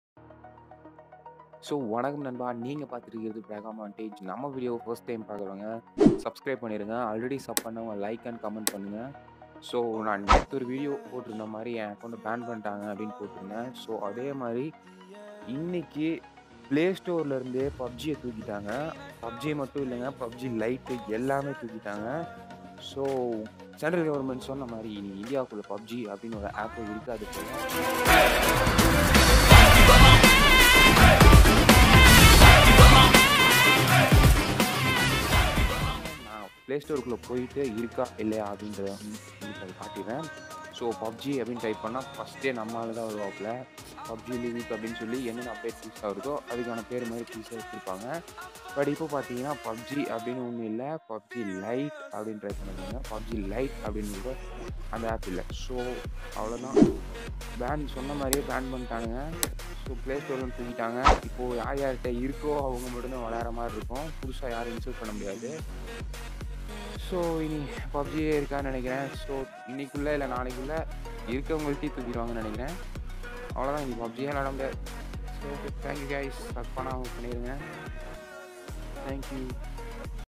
PUBG MOBILE REMOVED FROM PLAY STORE TODAY & PUBG LITE ALSO || FIRST STEP OF PUBG BAN IN INDIA
About Video :-
  Hello Friends welcome back another new video this video is show about pubg mobile ban news in india pubg mobile official banned India...